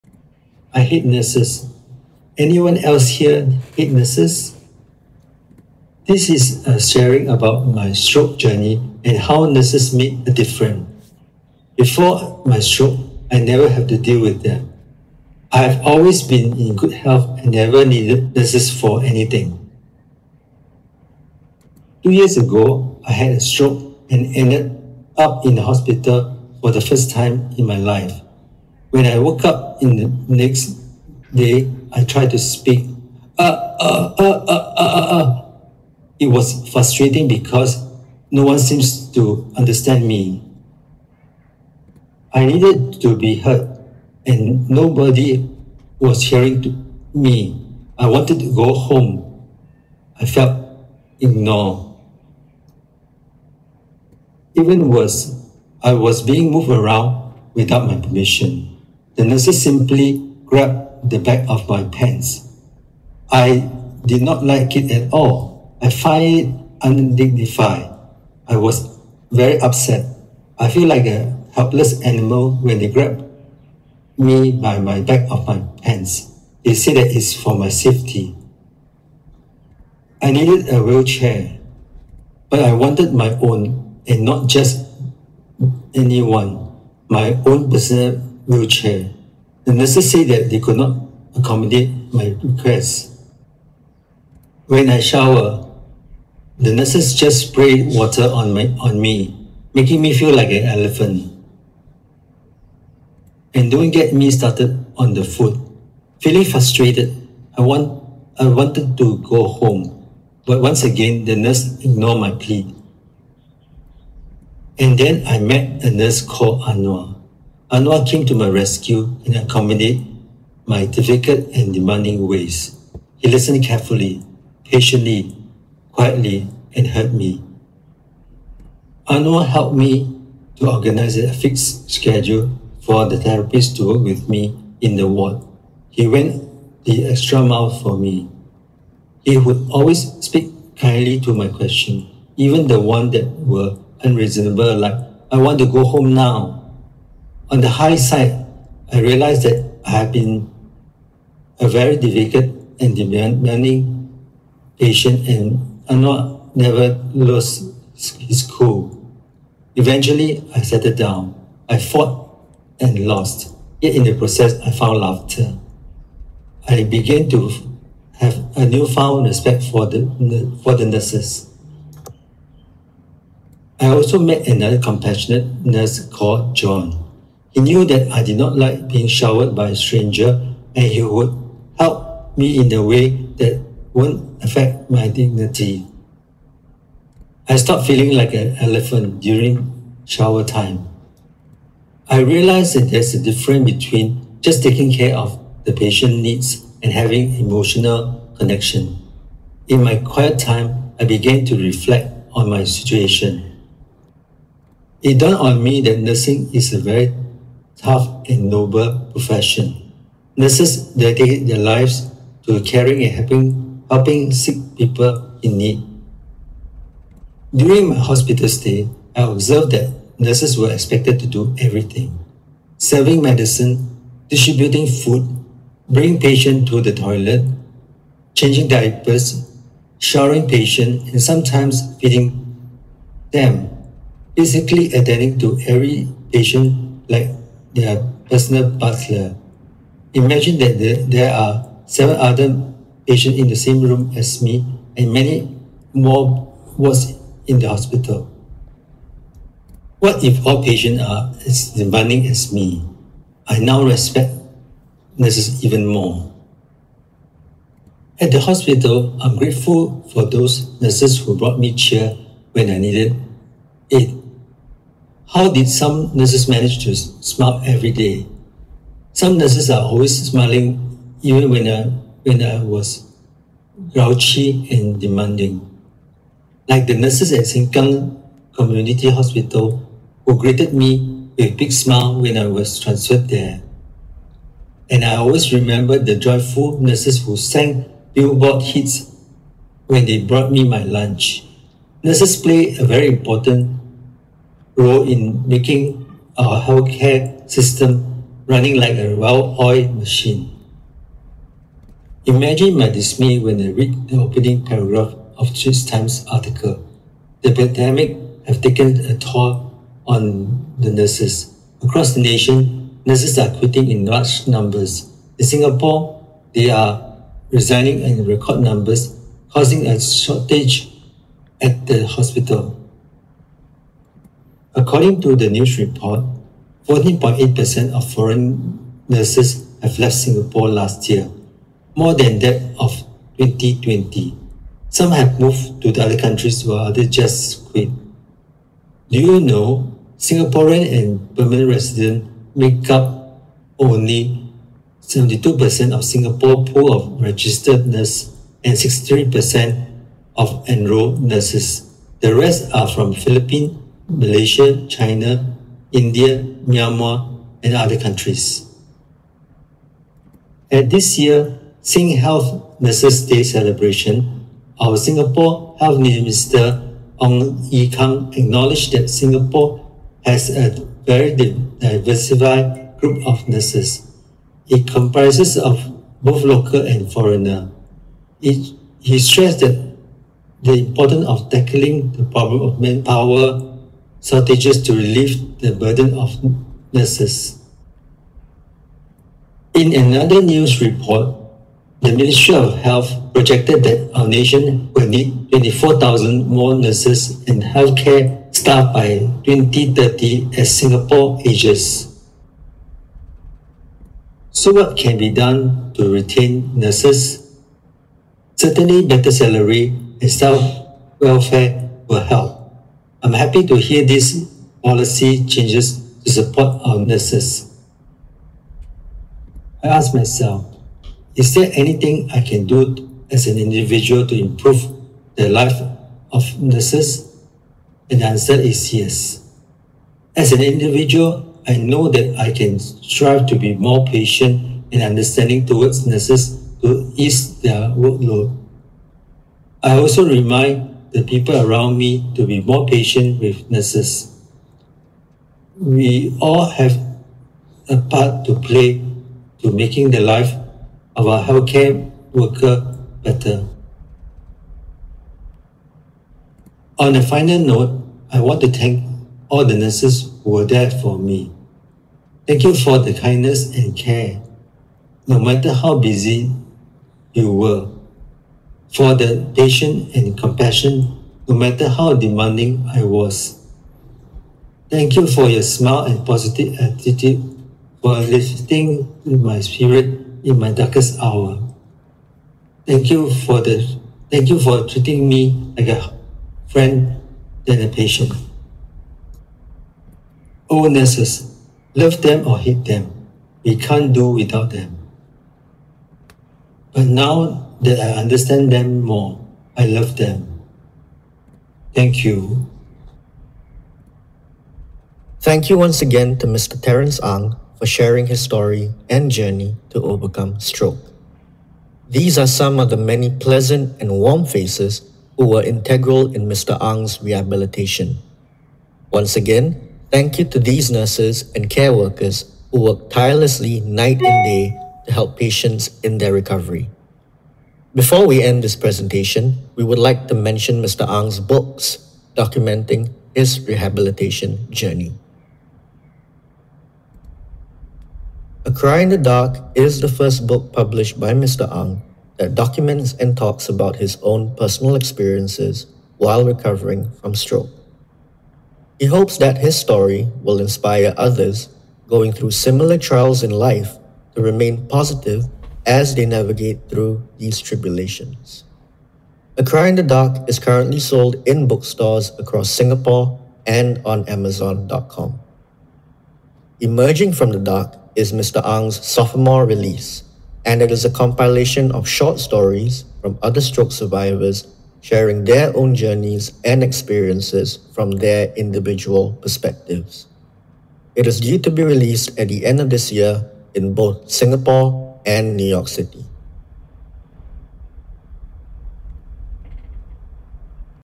My Stroke Journey | How Nurses Made a Difference | 14th World Stroke Congress
My stroke journey - How nurses made a difference by Terence Ang